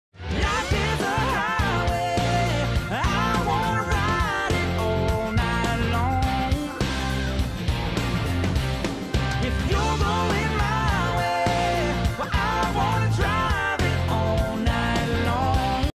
*VIEWER DISCRETION ADVISED* Guy Gets Hit By Car
Horrifying footage of innocent New York man crossing road but what happens next is shocking...